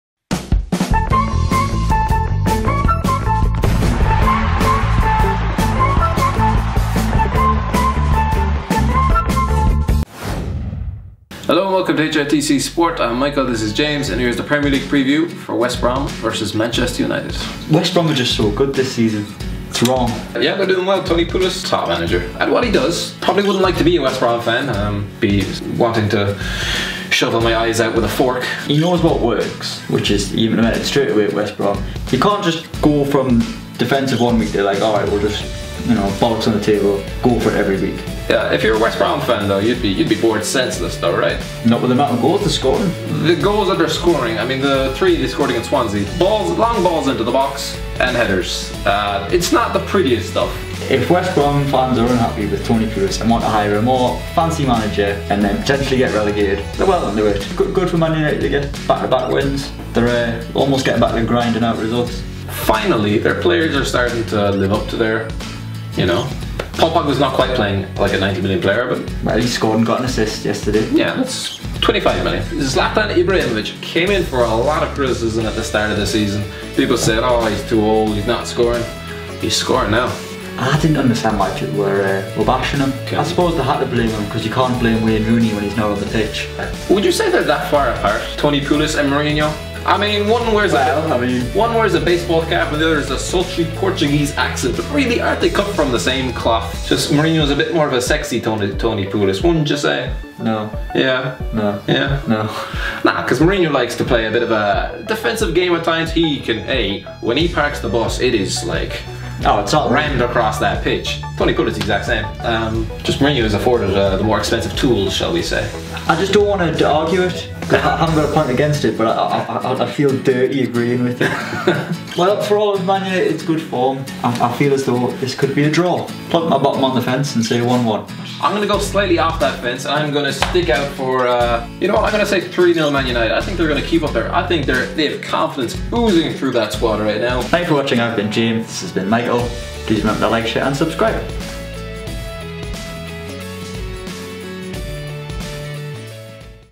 Are Mourinho And Pulis Similar? West Brom vs Manchester United Preview
Saturday's Premier League action sees West Brom entertain Manchester United. Michael and James preview the clash, including an interesting comparison between Mourinho and Pulis.